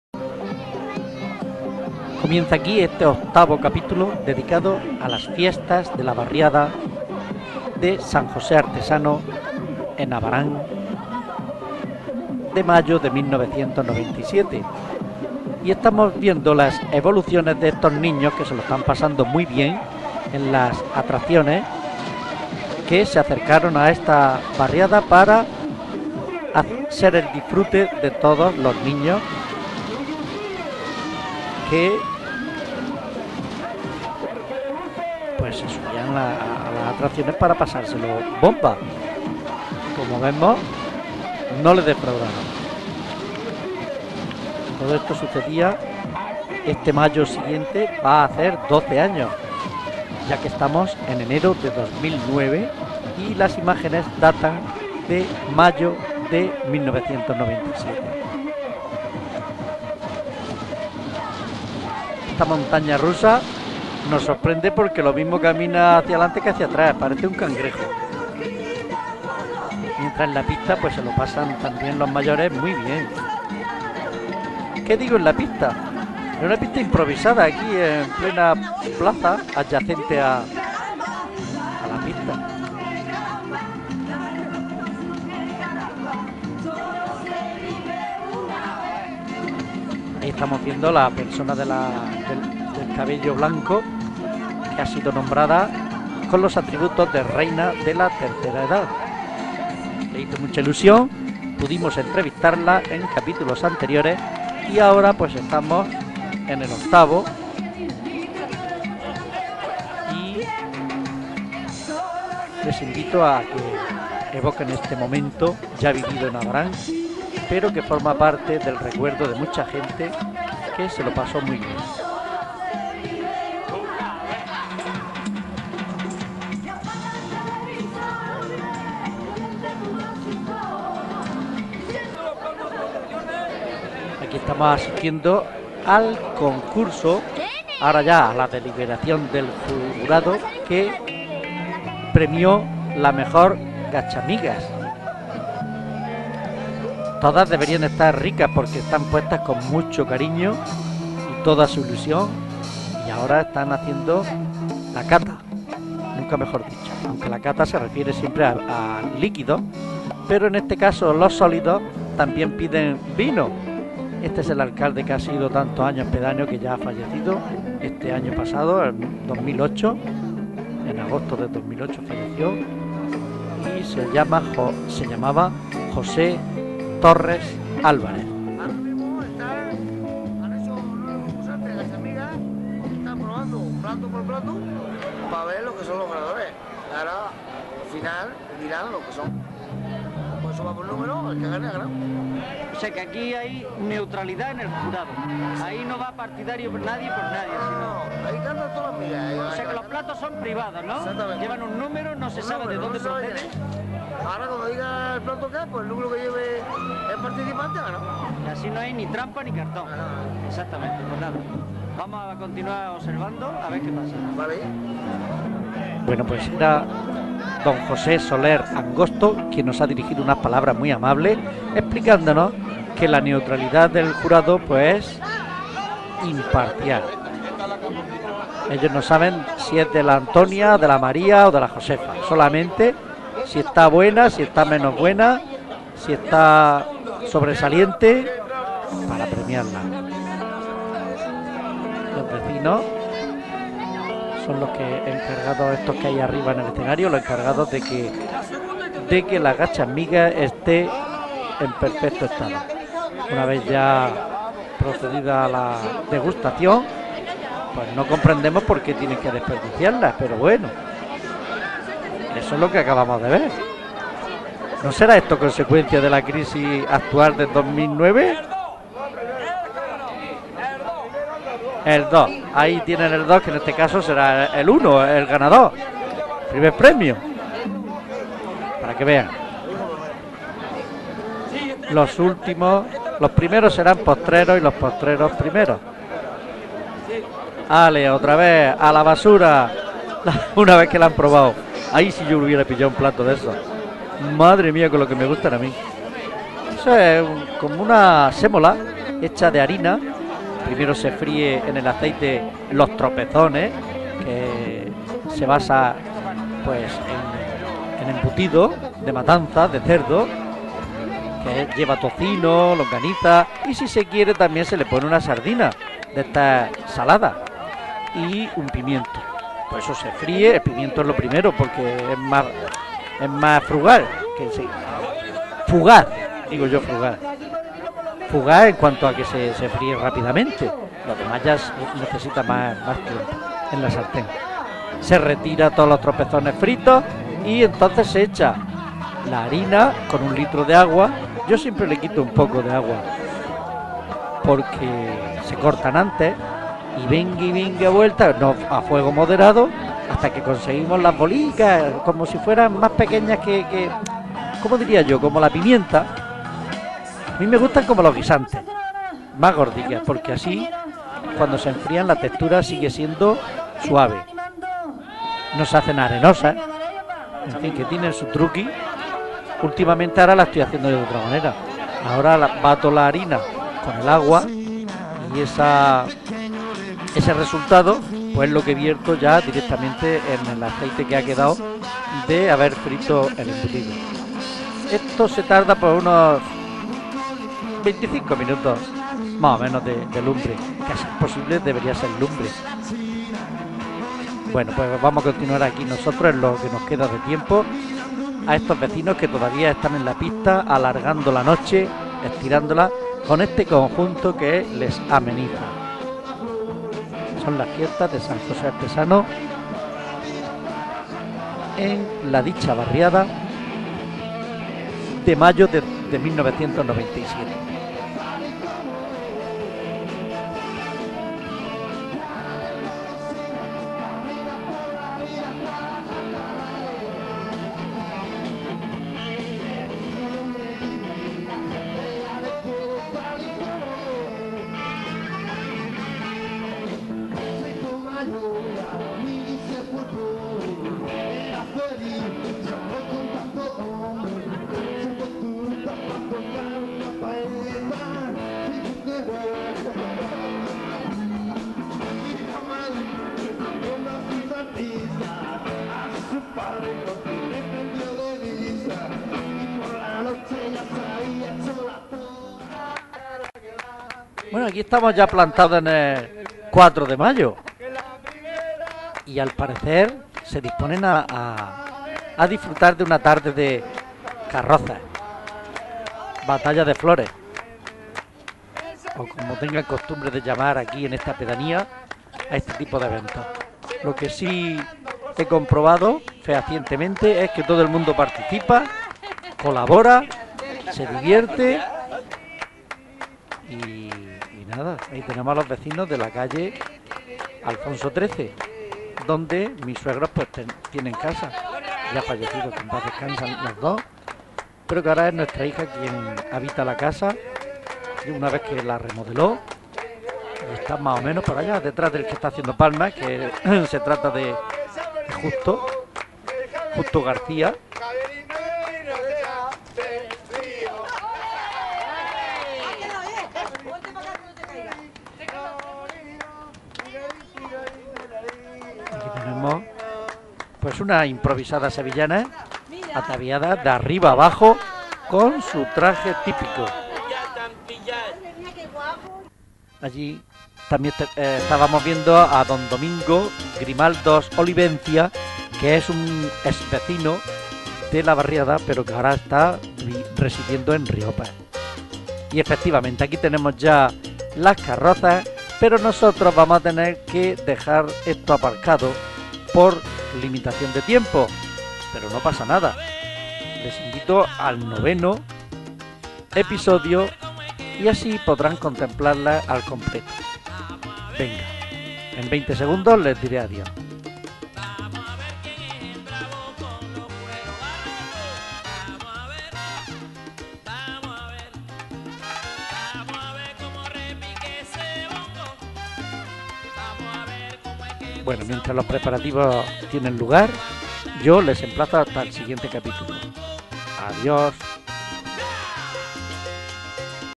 Ángel López Miñano (Abarán) 8ª Noche Gachasmigas Fiesta San José Artesano 97
La noche de las gachasmigas se ha implantado en innumerables lugares, también esta barriada de San José Artesano en este mayo de 1997. Me contaron el funcionamiento del jurado para dar el primer premio y vimos a los vecinos disfrutar de lo lindo.